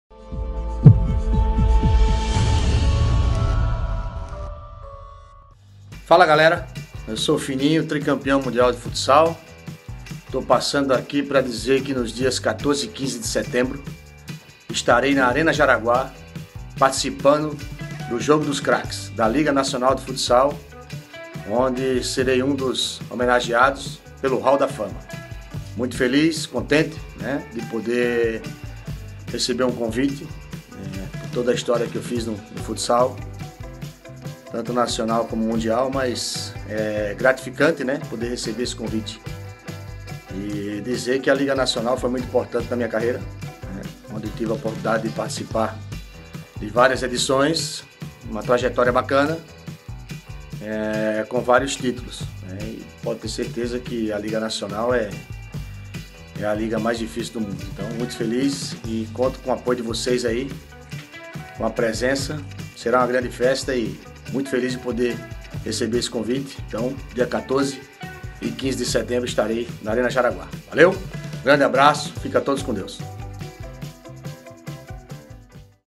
Hall da Fama homenageará Fininho
Fininho, ídolo do nosso futsal, será homenageado no Hall da Fama da LNF e será um dos capitães no Jogo dos Craques